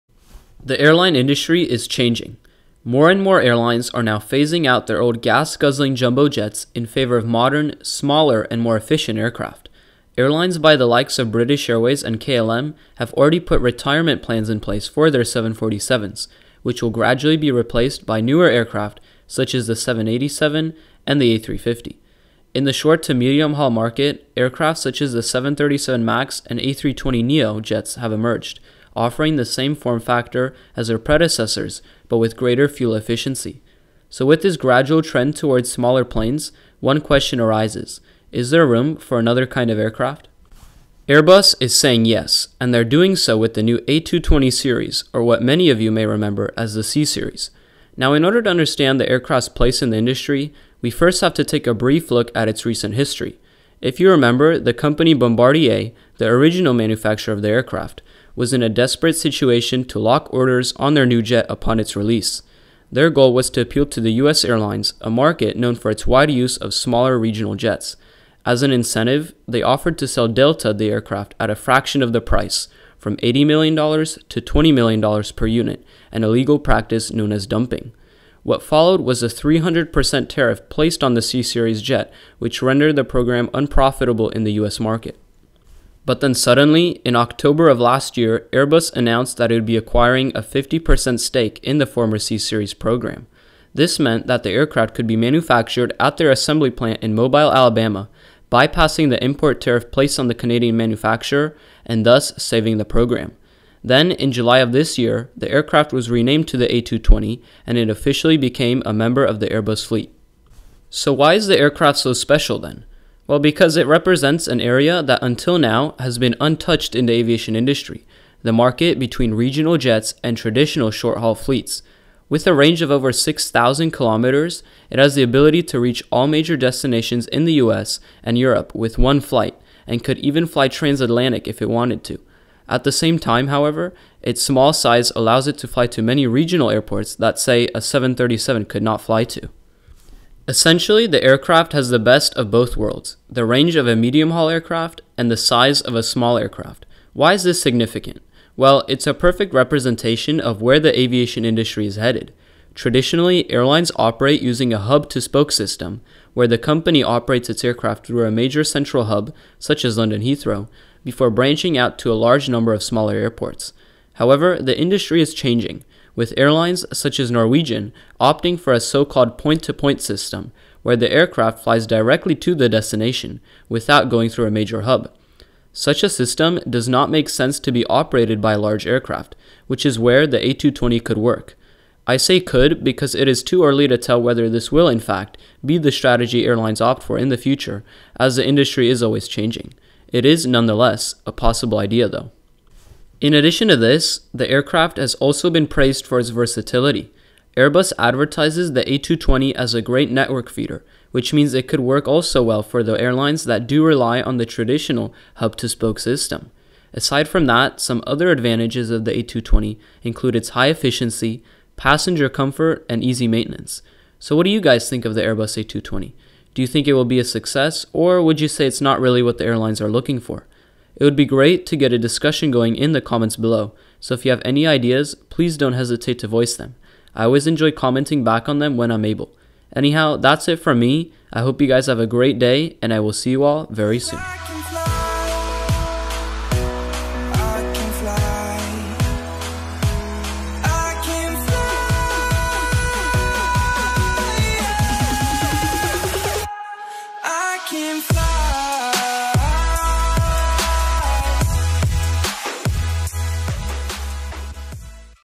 Why the AIRBUS A220 Could Be the Perfect Aircraft
• AviationToday Series

Hey guys, thanks for tuning in to another episode, it's great to have you on the channel. Today we discuss the Airbus A220, it's place in the industry, and if it could potentially be the perfect aircraft for airlines. Enjoy!

Apple Maps
Airbus

• Created using iMovie
• Pure Aviation 2018